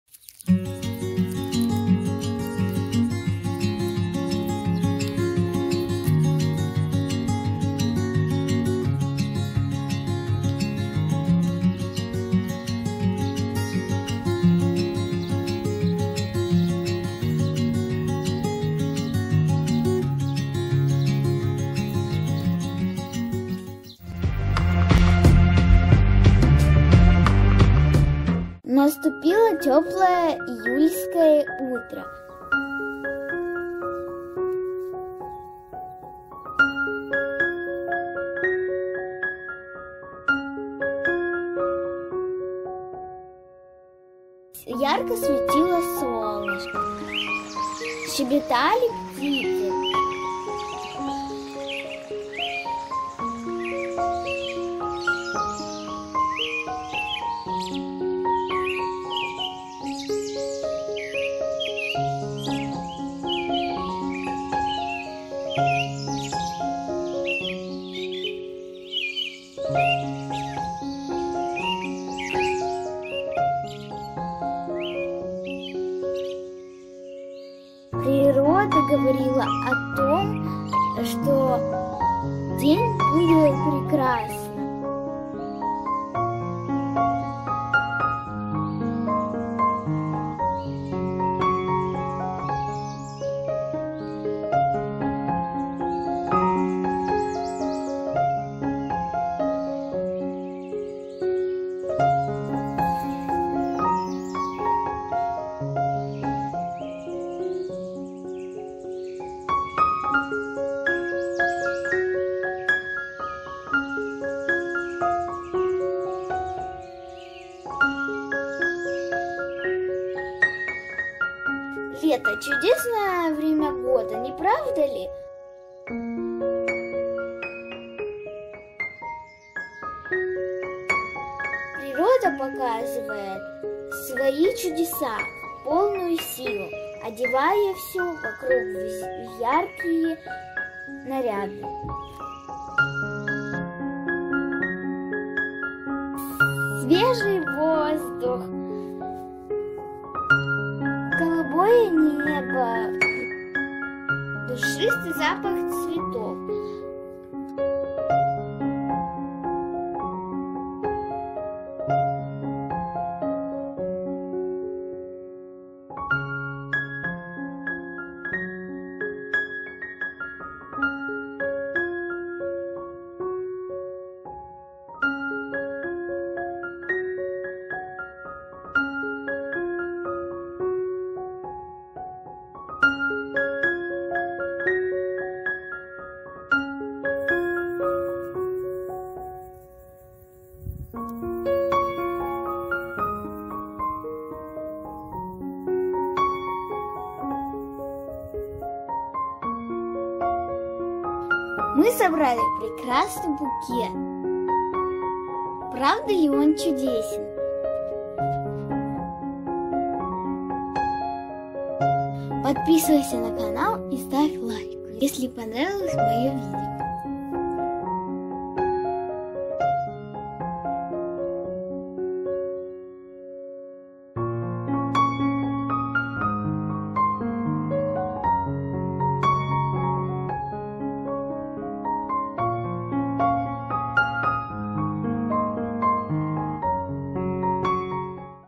VLOG Мой летний день/My Summer day.
Summer#Jule#my day#
VLOG: Всем приветик! В этом видео, Вы увидите красивое летнее утро!